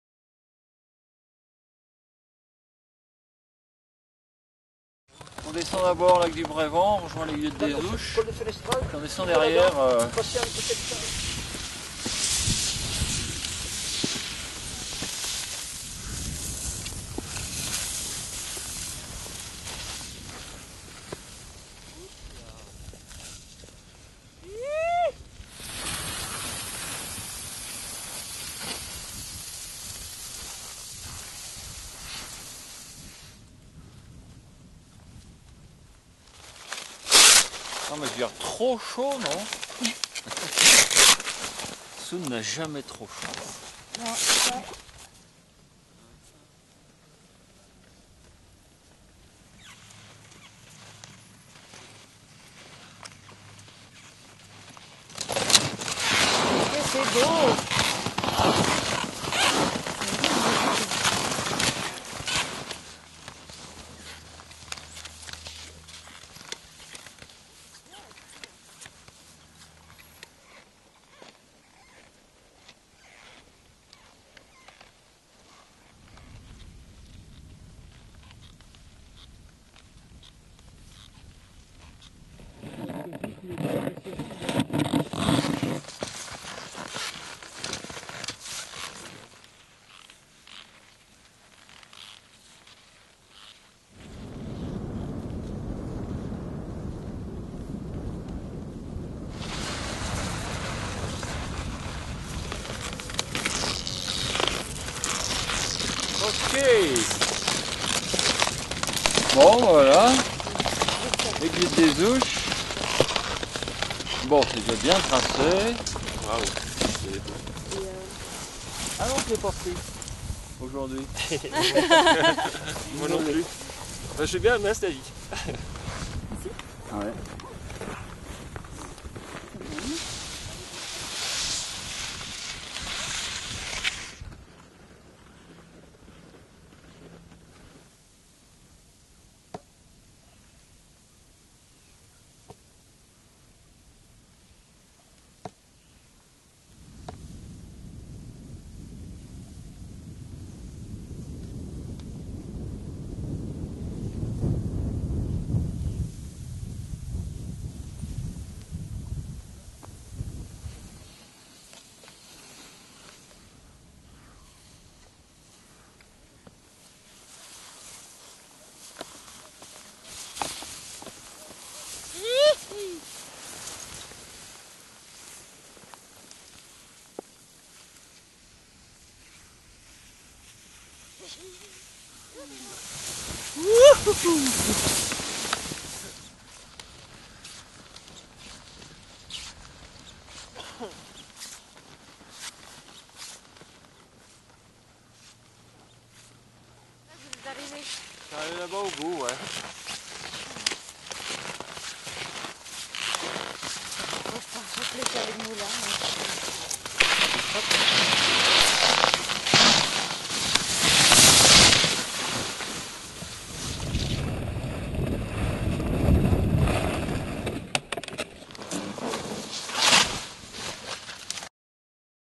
Aiguillette des Houches via le Brévent Aiguilles Rouges Chamonix Mont Blanc ski de randonnée
Mars 2012, ski de randonnée, montagne... Départ de Brévent dans la vallée de Chamonix Mont-Blanc et direction l'Aiguillette des Houches dans les Aiguille Rouges... Merci à Michel Bordet.com à Vincent Devalkener et Sunanda Gérard...